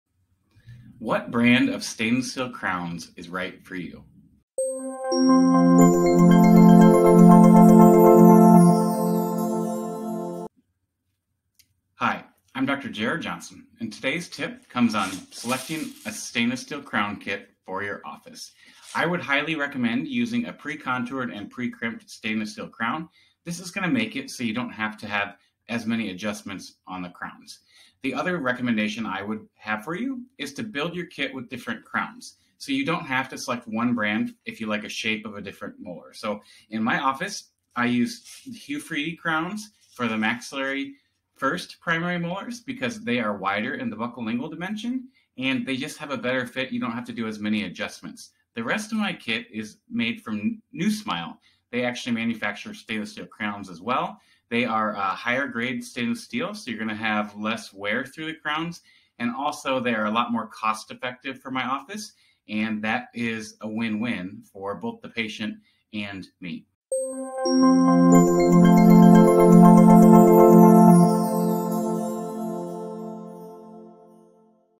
What is the best SSC brand?
What type of SSC kit is the best for your office?  I recommend using a precontoured, precrimped SSC.  This will reduce chair time and frustration when placing SSCs.  There are a wide variety on the market, and you don't have to have a kit with "one brand."  In fact I use two brands at my office.  I recommend HuFriedy crowns for the primary maxillary first molars, and the rest any brand will do.  I use NuSmile SSCs as they are a higher grade of stainless steel and will have less wear from grinding, and they are more cost effective.  If you do a lot of these I recommend buying in bulk as they don't have an expiration date.  Have a great Monday!